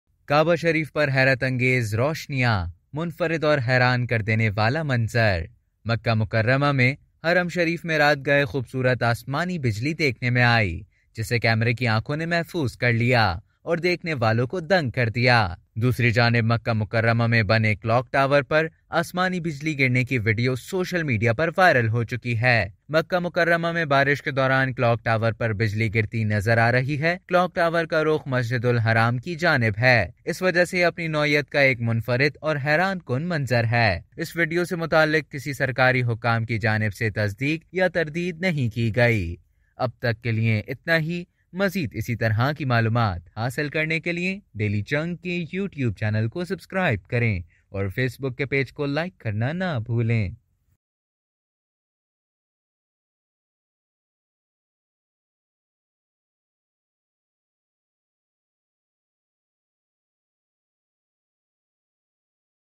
Video Viral On Social Media For Lightning Strikes In Saudi Arabia Makkah
The video of lightning striking the clock tower in Makkah has gone viral on social media.